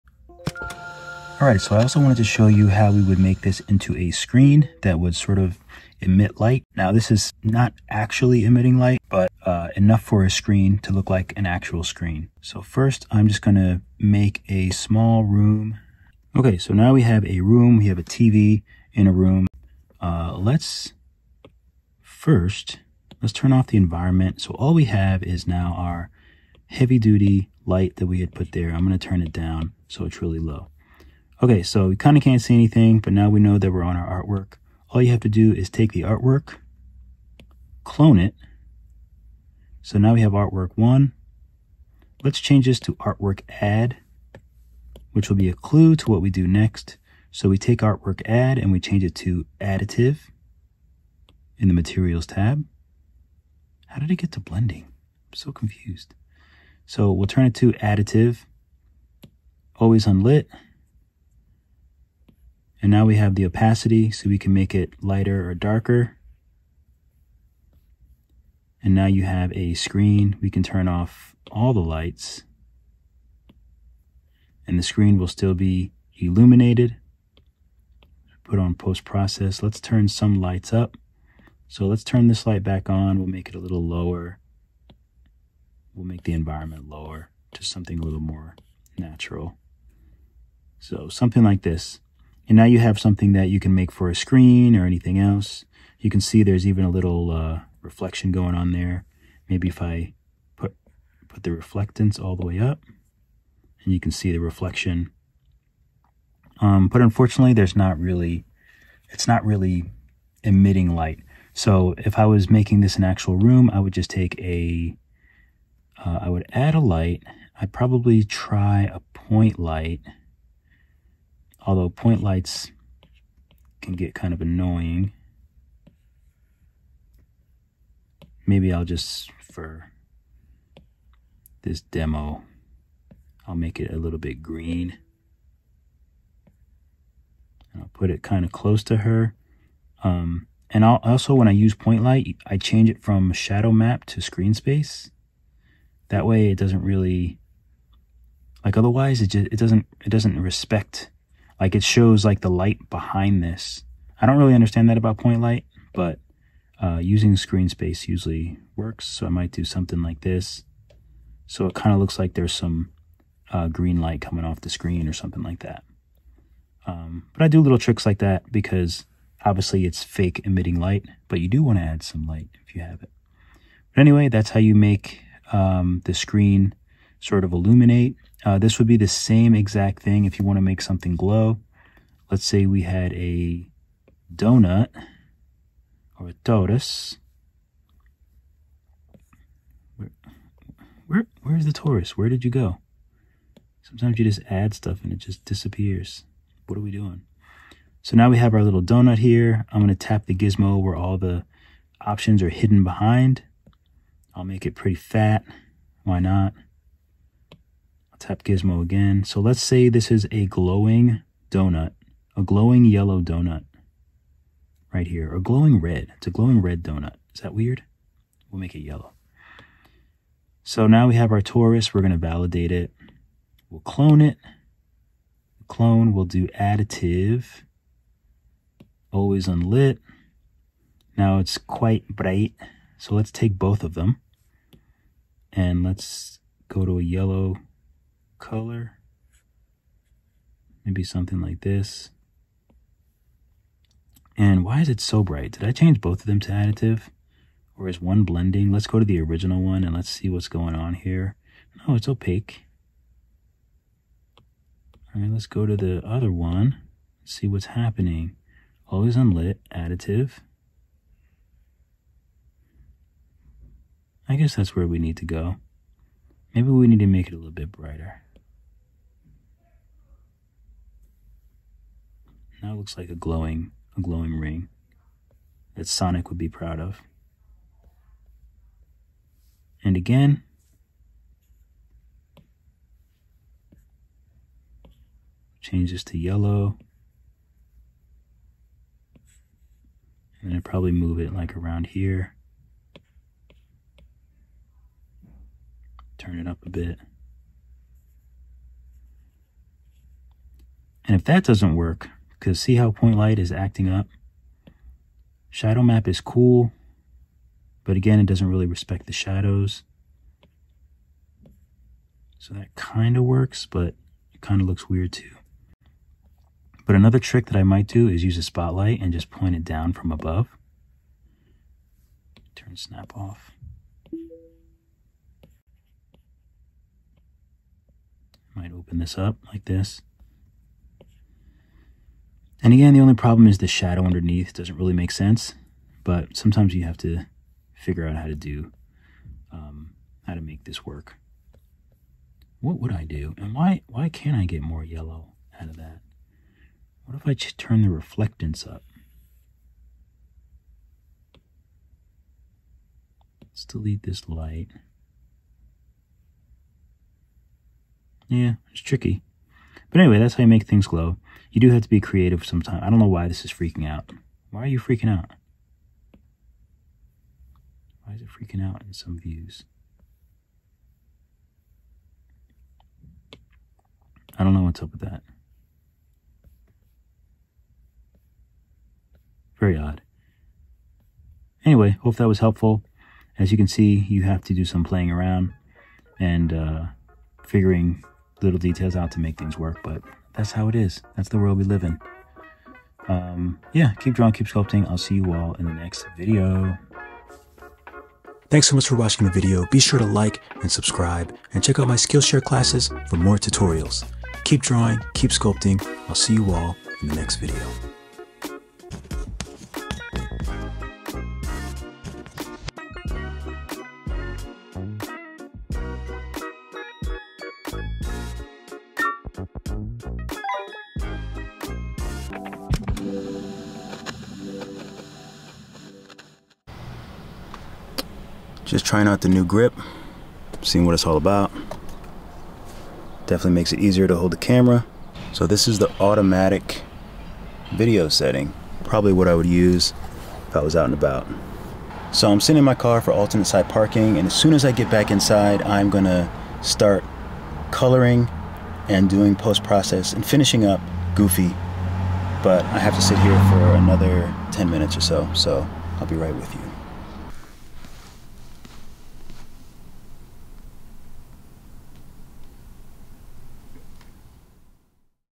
Make the Screen Light up 💡 | Nomad Sculpt 1.76 Quick Tutorial
Should have added this to the last video, here's how to make the screen become "Illuminated"(It does not emit very much practical light though)
Adding a 2D image to 3D objects: Great for photos, or TV Screens!
Hope this helps Han! Enjoy!